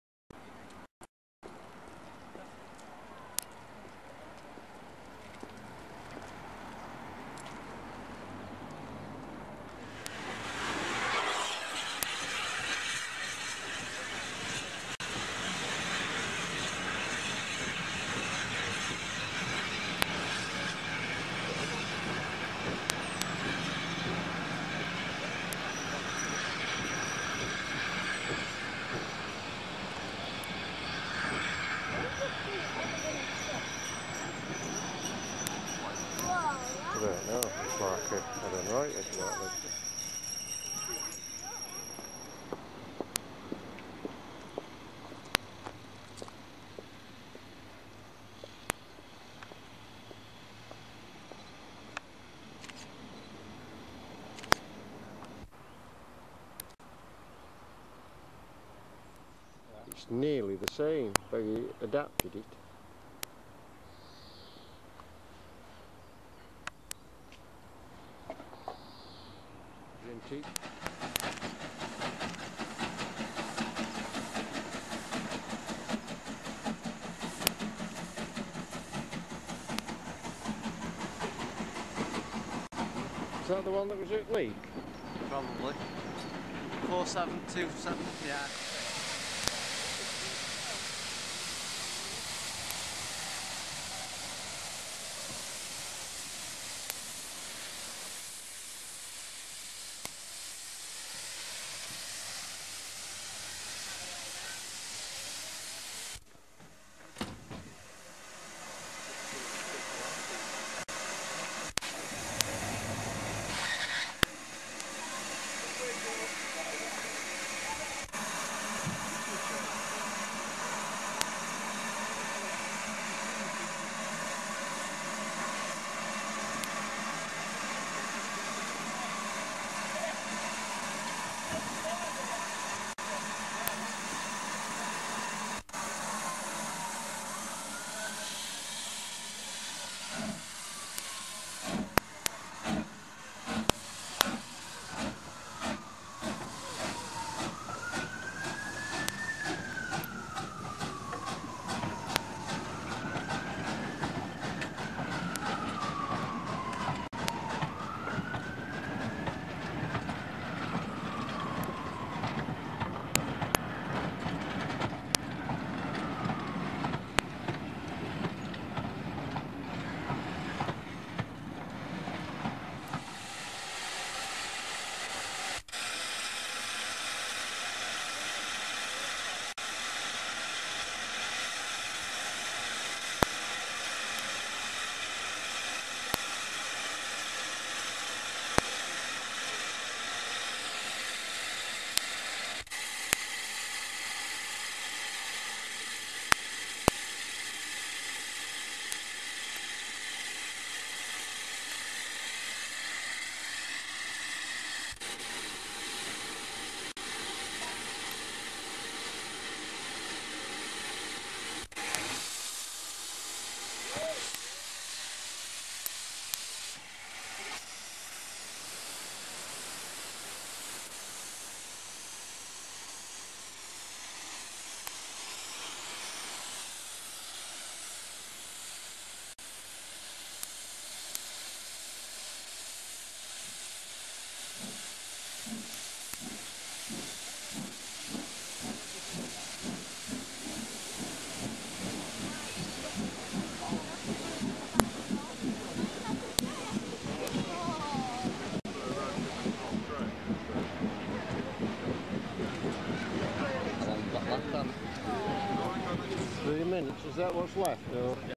Keighley And Worth Valley Railway (2009)
Footage in this video was found on some of my old JVC VHS-C camcorder tapes. So in this video then, I spend a day at the Keighley and Worth Valley railway. This footage was taken during 2009